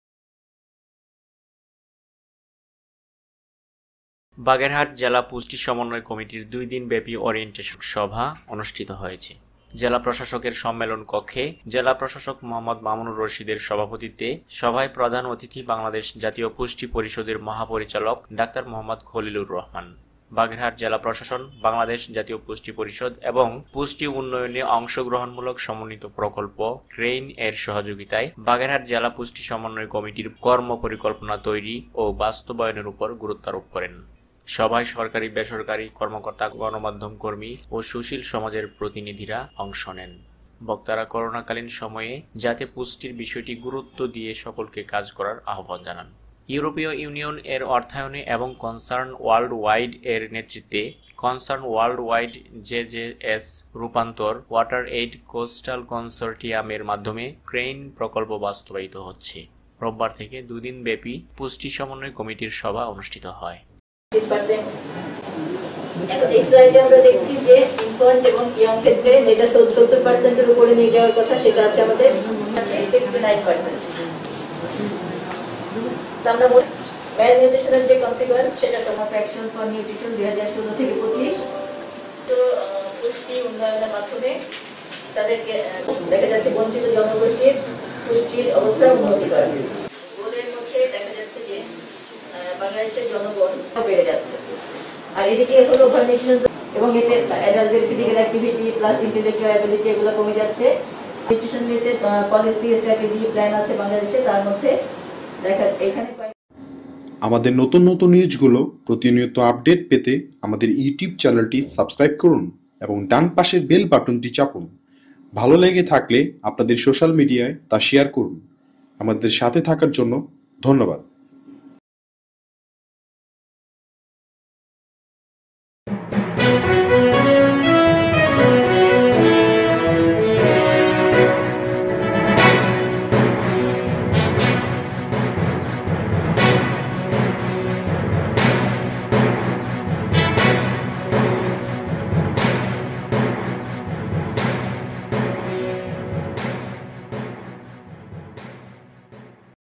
পুষ্টি সমন্বয় কমিটির ওরিয়েন্টেশন অনুষ্ঠিত || Orientation of Nutrition Coordination Committee held
বাগেরহাট জেলা পুষ্টি সমন্বয় কমিটির দুই দিন ব্যাপী ওরিয়েণ্টেশন সভা অনুষ্ঠিত হয়েছে।   জেলা প্রশাসকের সম্মেলন কক্ষে জেলা প্রশাসক মোঃ মামুনুর রশীদের সভাপতিত্বে সভায় প্রধান অতিথি বাংলাদেশ জাতীয় পুষ্টি পরিষদের মহাপরিচালক ডাঃ মোঃ খলিলুর রহমান।